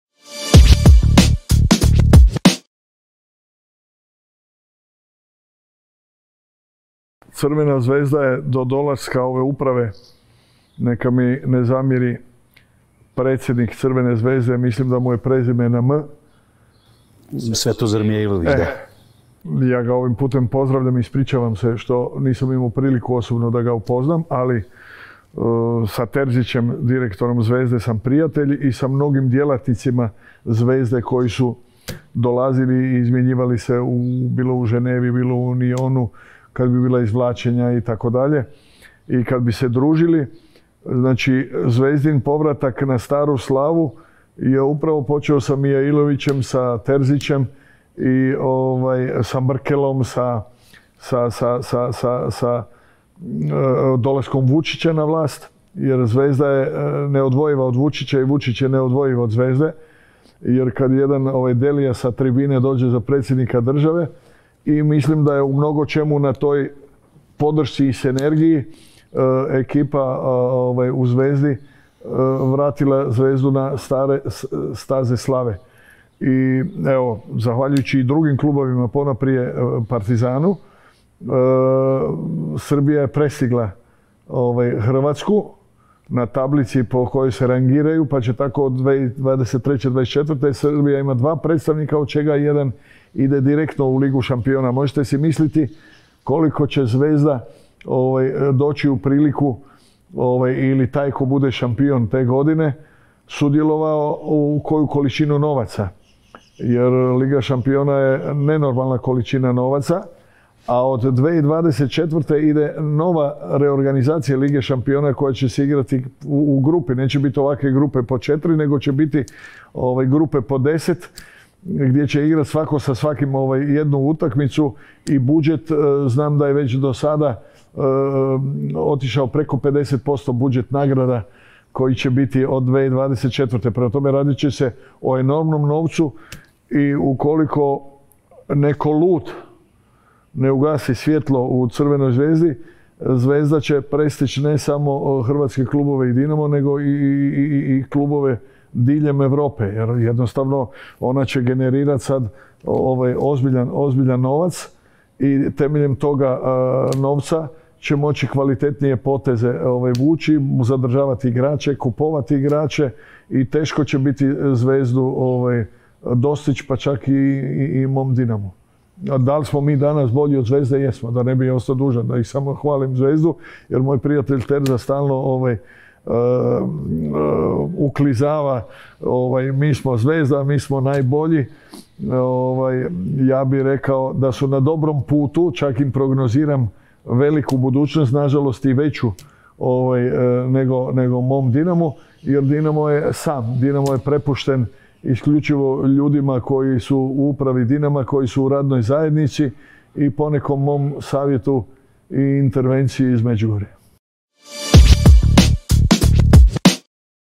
"Uskoro će biti teško dostići Zvezdu, čak i mom Dinamu": Zdravko Mamić EKSKLUZIVNO za Sportal
Zdravko Mamić govorio je o razlozima uspeha Crvene zvezde u evrokupovima prethodnih sezona, ali i celokupnoj situaciji u srpskom klupskom fudbalu.